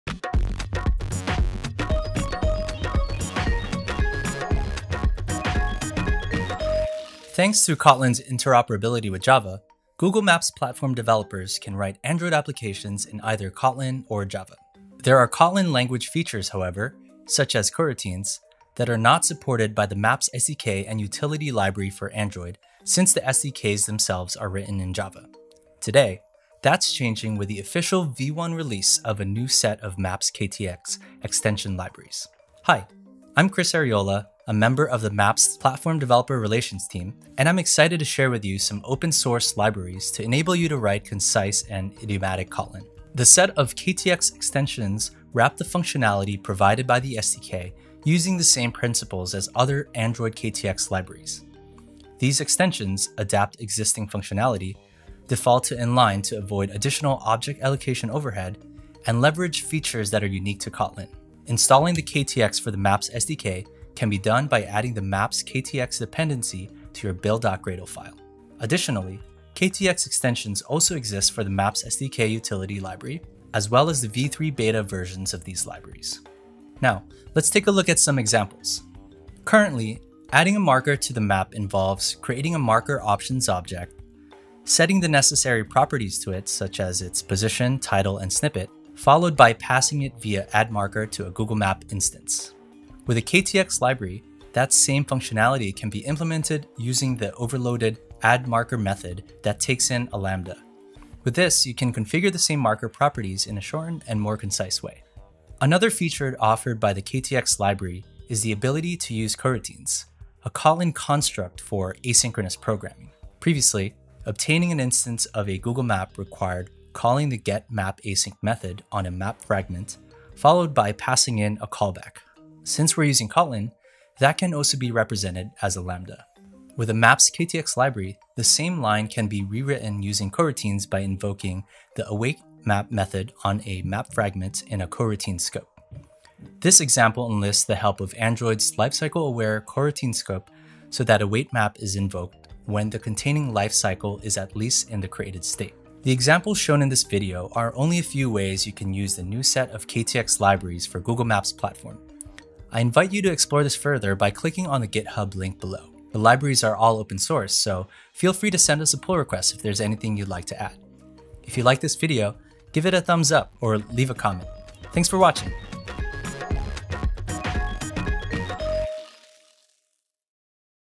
Concise Kotlin Android development with Maps KTX
With the official release of the new Maps KTX extension libraries, Google Maps developers can now use Kotlin language features — such as extension functions and coroutines — to build Android applications. In this episode, Chris Arriola goes over how you can write concise and idiomatic Kotlin with Maps KTX extension libraries.